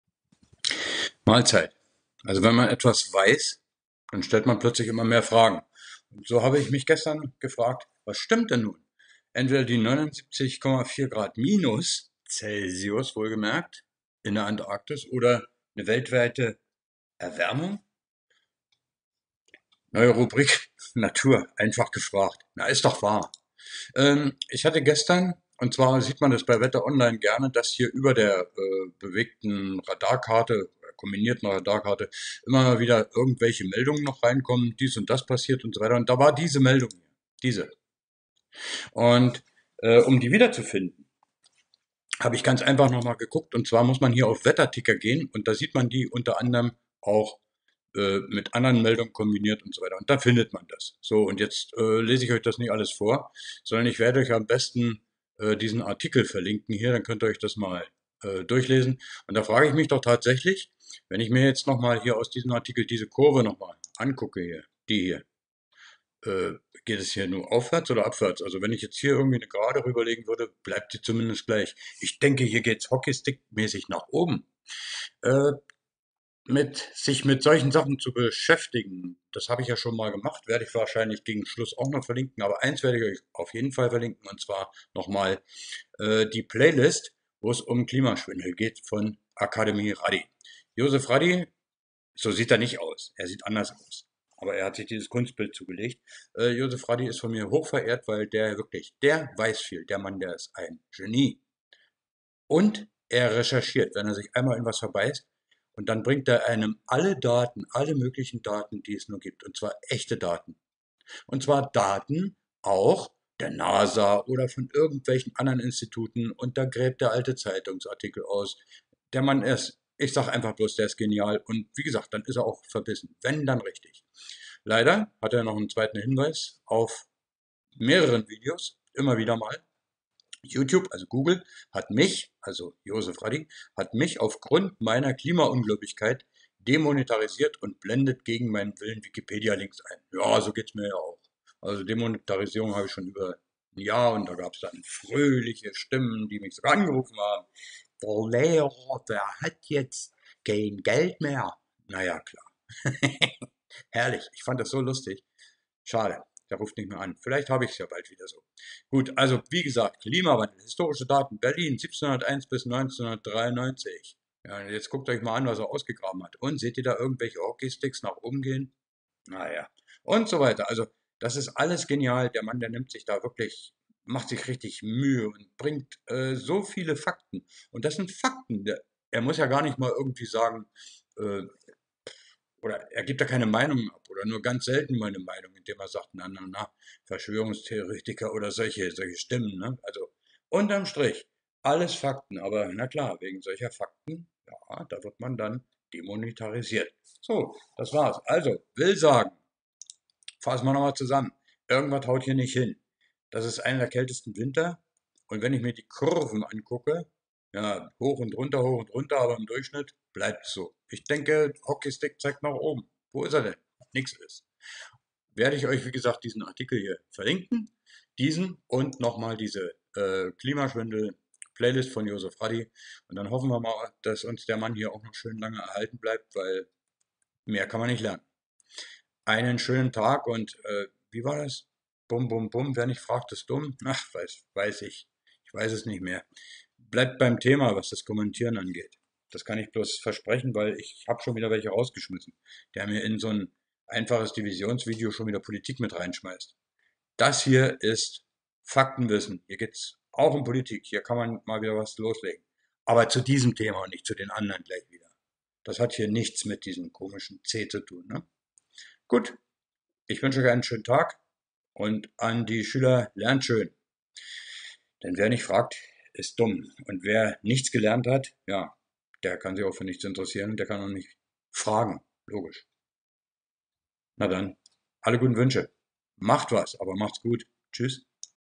Was stimmt denn nun?! Fast minus 80°C in der Antarktis oder die weltweite Erwärmung?
+++ Meinungsfreiheit im MaPhy-Forum +++ Wer nicht fragt, bleibt dumm. UND: Man kann nur fragen, wenn man etwas weiß. Hier der Minus-80-Grad-Artikel

Björn Köhler